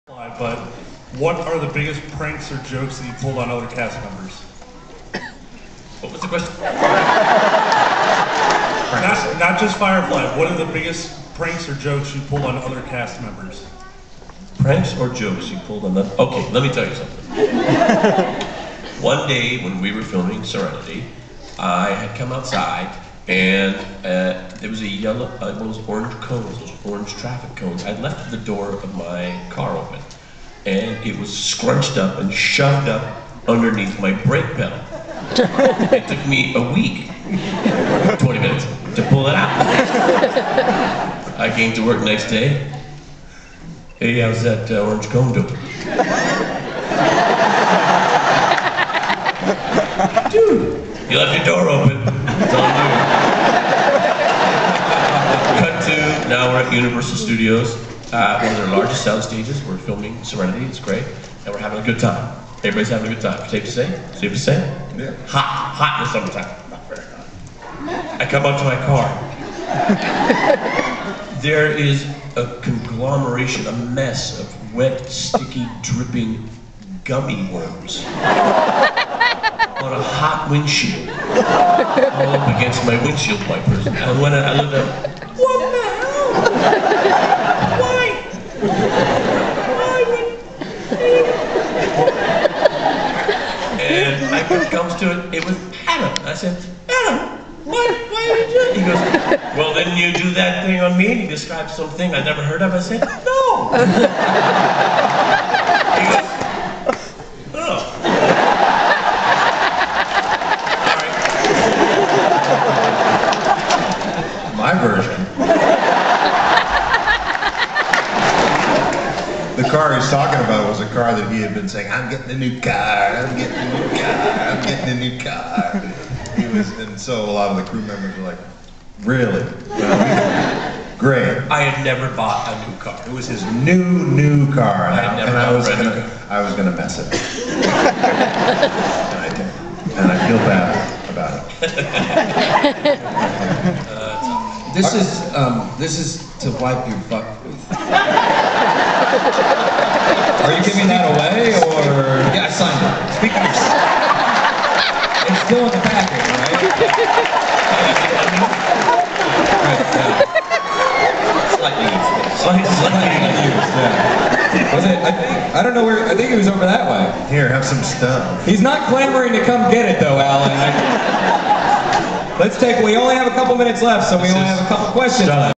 Firefly Panel - St. Louis Comic Con - Pranks
Nathan and Adam discuss on set pranks while filming "Serenity"